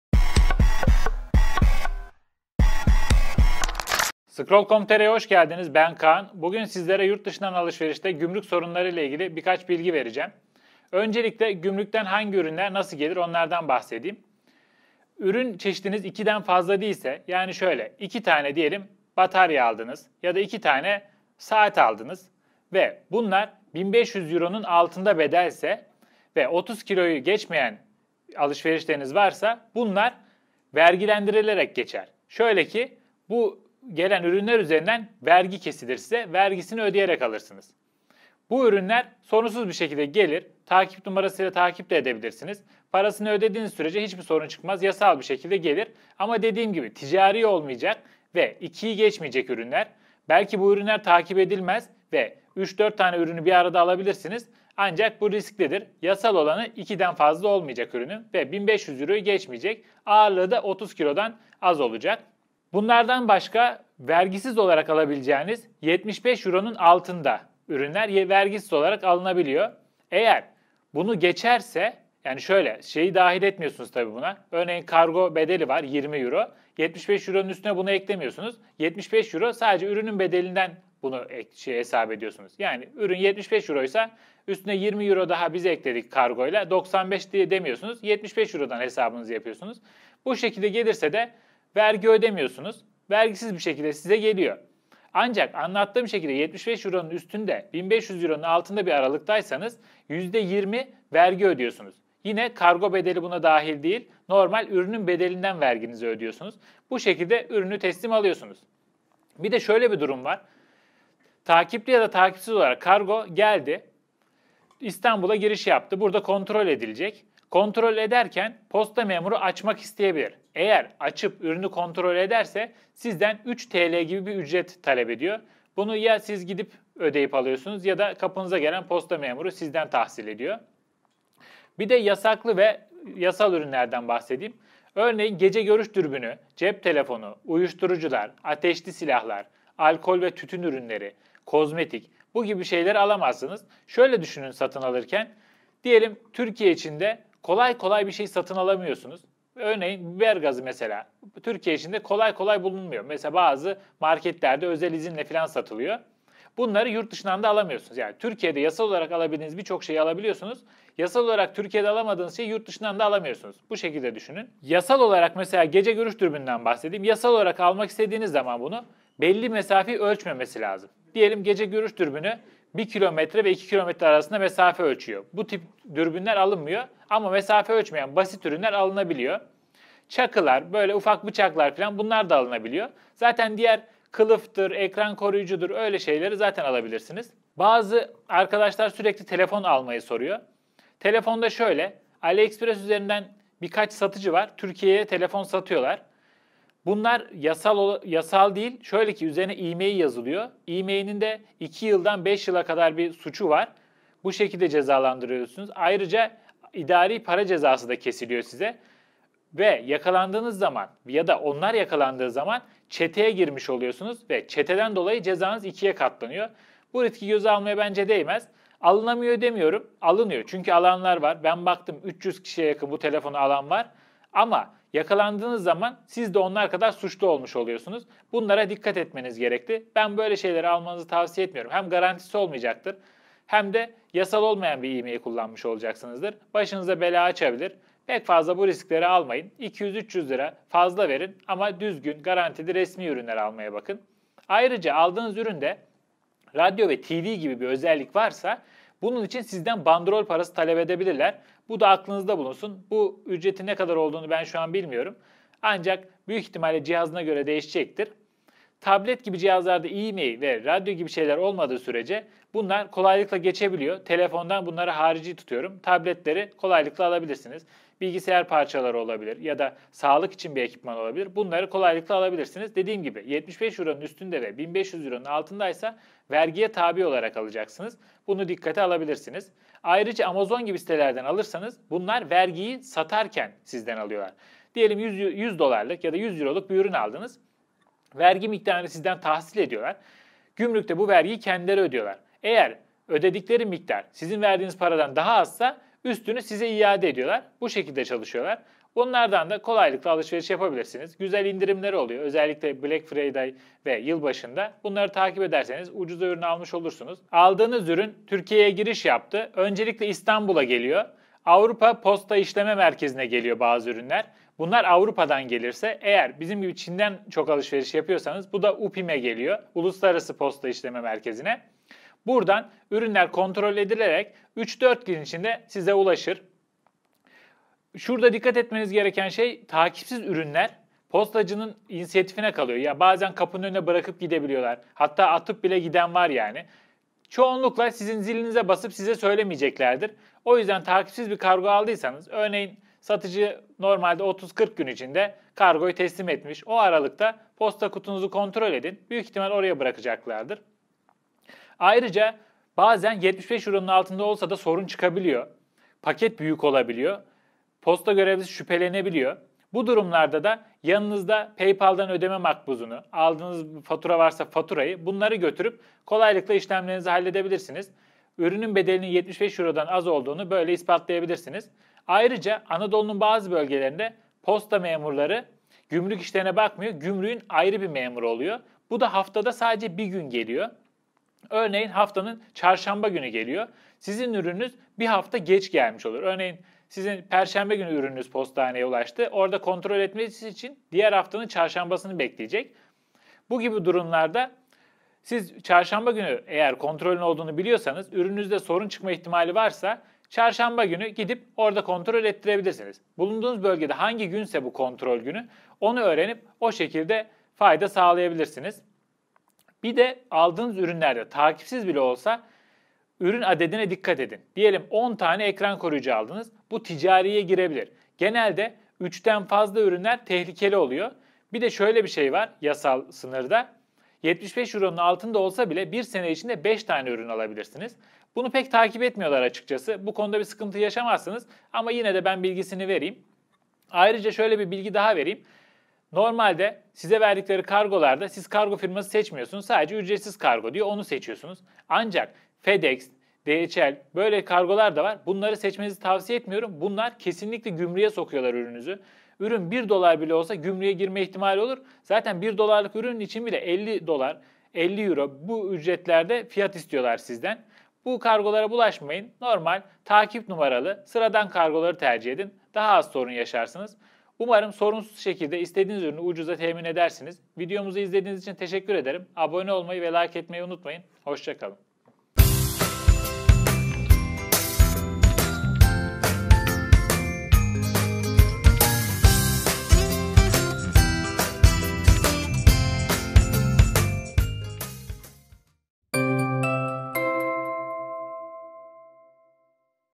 Yurtdışından Alışverişte Gümrük İşlemleri
Merhaba sevgili Scroll izleyenleri, bu videomuzda yurtdışından yaptığımız alışverişlerde yaşayabileceğimiz sıkıntıları ve nelere dikkat etmemiz gerektiğiyle ilgili bilgileri paylaştık. Keyifli seyirler :)